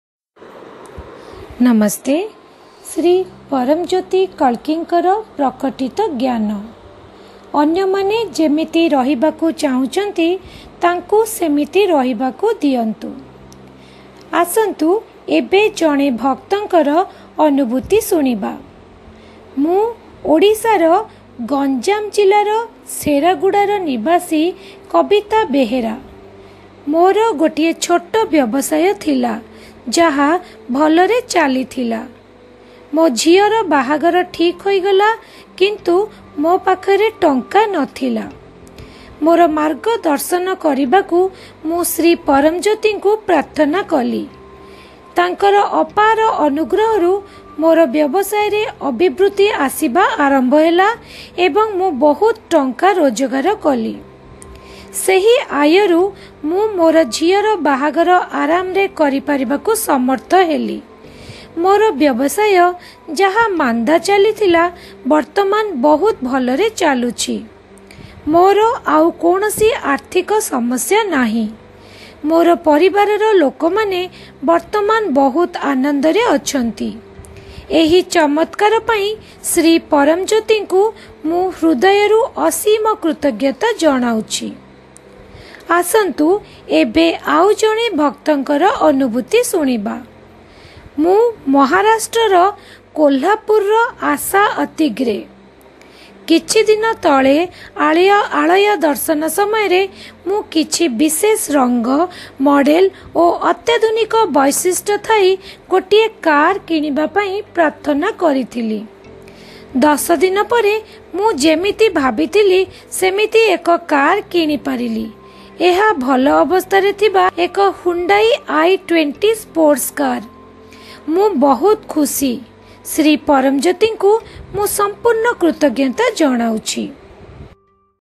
Odia - A prayer done in helplessness opened up doors of grace.
Miracles are everyday affair in lives of devotees.